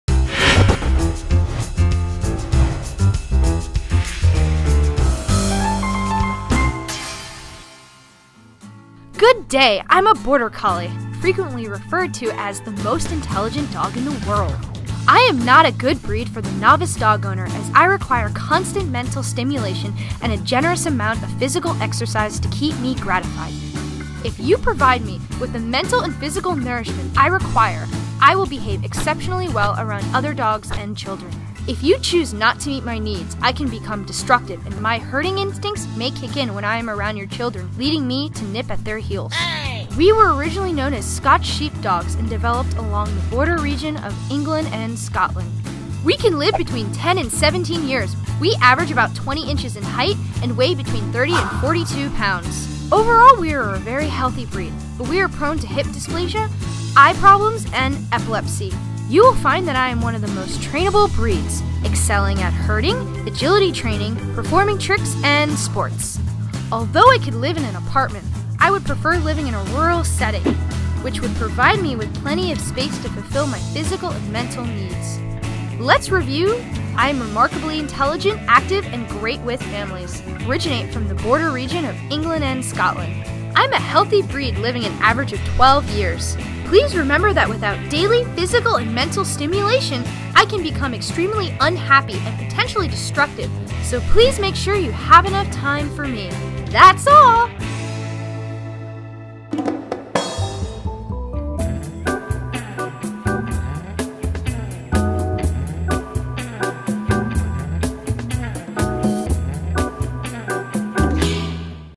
Doglopedia - Border Collie
I'm a Border Collie.
Google+: gplus.to/thepetcollective"

Welcome to Doglopedia, your resource to learn all about different breeds! Here's a quick, fun overview of the Border Collie, hosted by ME, the Border Collie. That's right, this time I get to train you! Happy petting!